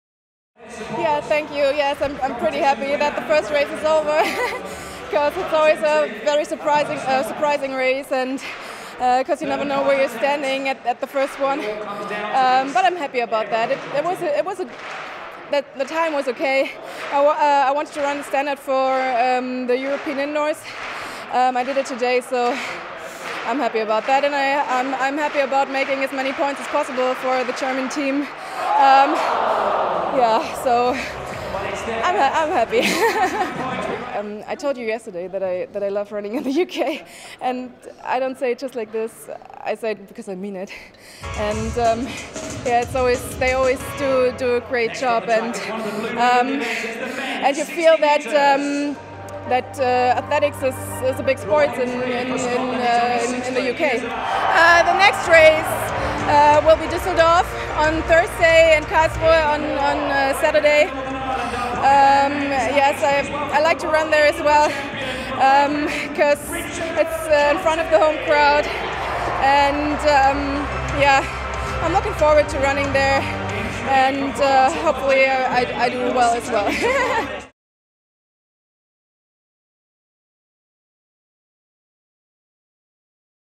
Glasgow International Match: Verena Sailer victorious in season opener
Germany's Verena Sailer saw off GB's Rachel Johncock to claim victory over 60m in her first indoor race of the season.
The sprinter clocked 7.27secs to take the win and spoke to Sportsbeat to look ahead to this spring's European Indoor Championships in Prague.